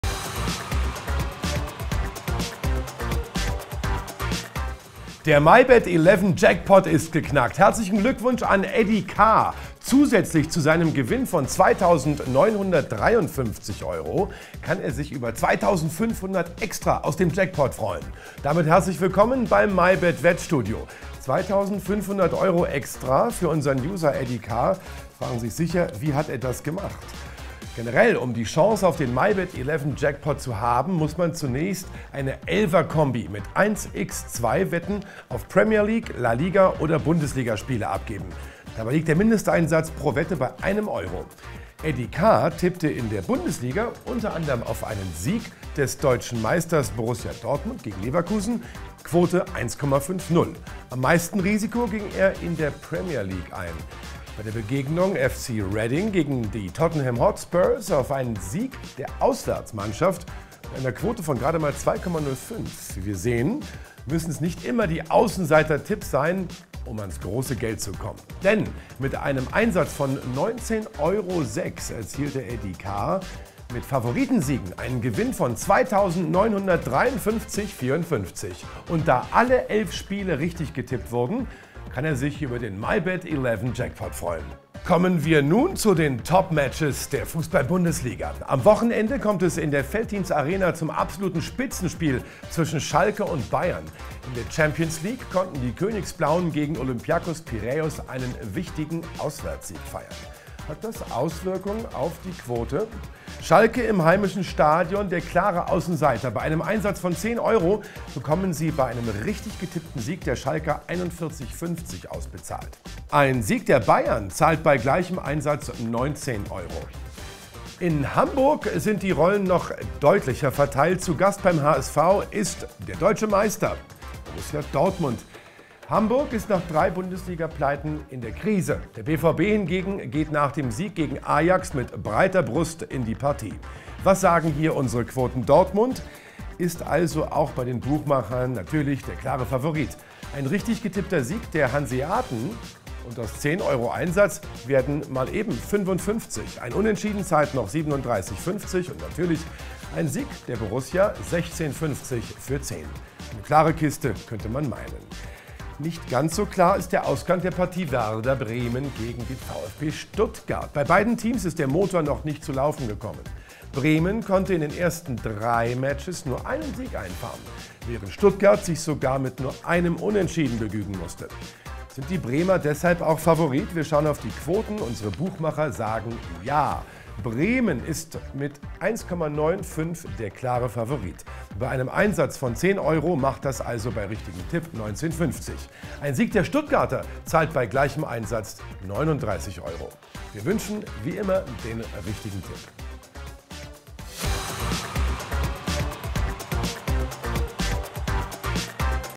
Mybet Bundesliga Wett Show zum 4. Bundesliga-Spieltag 19.09.12
Die Mybet-Wettshow zum 4. Bundesliga-Spieltag. Mit dabei ist under anderem der Bundesliga-Klassiker zwischen dem FC Schalke 04 und dem FC Bayern München.